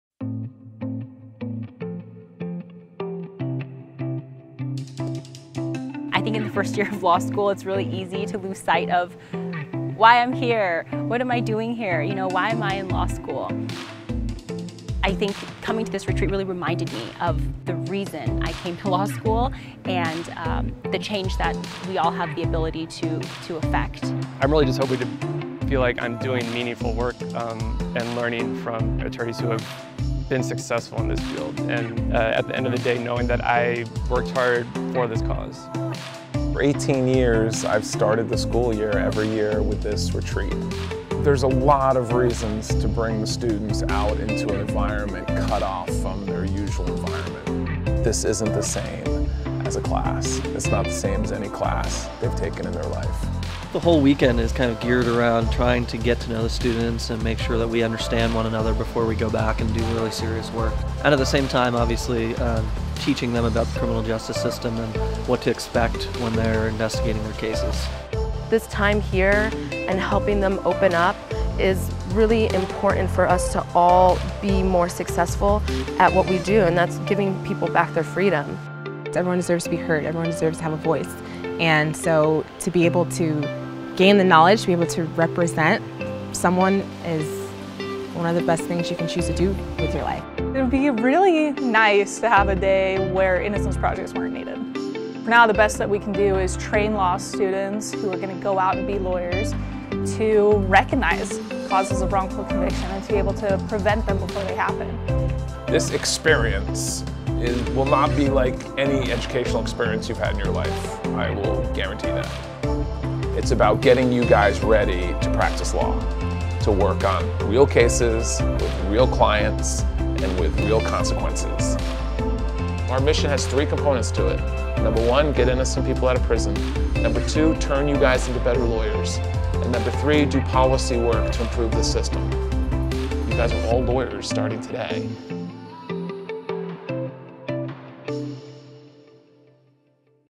California Innocence Project Interns Retreat
California Western School of Law students get a fresh outlook on the law learn about what goes into exonerating wrongfully convicted people during a retreat led by California Innocence Project Executive Director Justin Brooks.